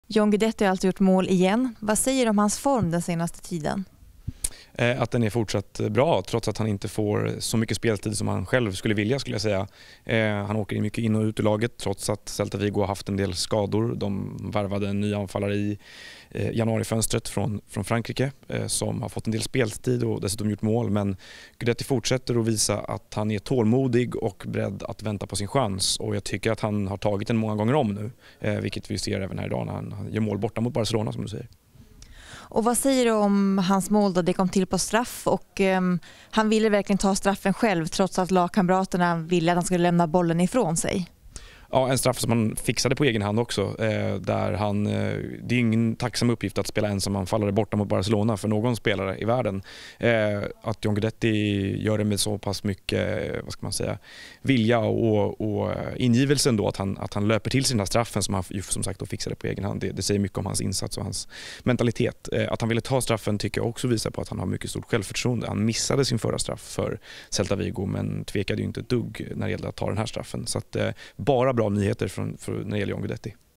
John Guidetti målskytt mot Barcelona
Han ordnade en straff, plockade själv upp bollen och stänkte in den i mål.